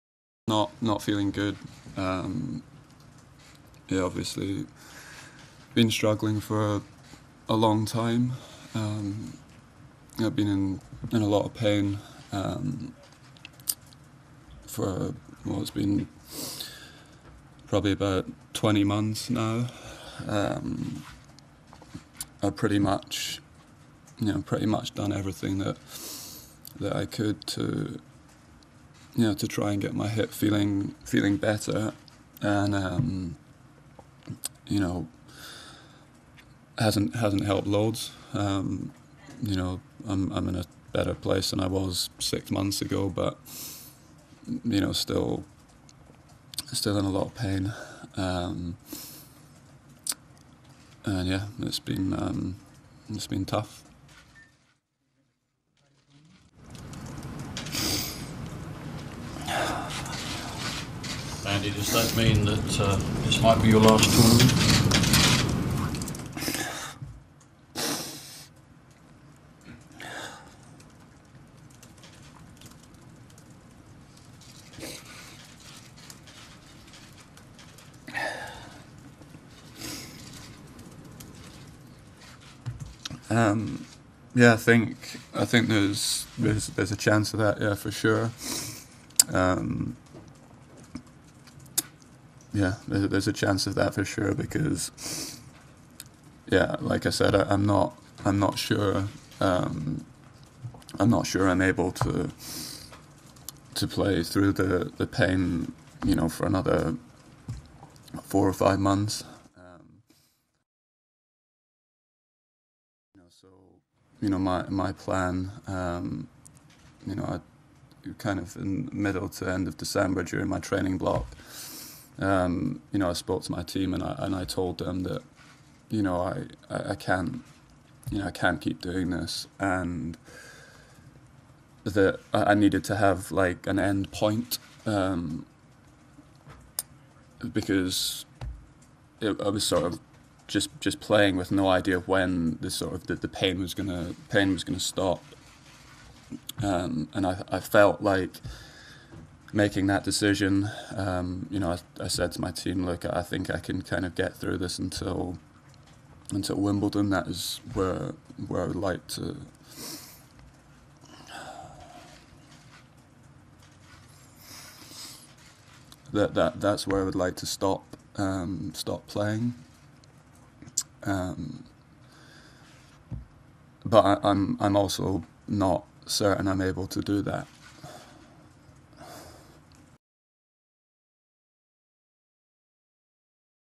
British tennis star Andy Murray to retire after Wimbledon
Video: During an emotional Australian Open press conference, Andy Murray struggled to hold back tears. The British tennis star has been struggling with a hip injury and is set to retire following Wimbledon tournament.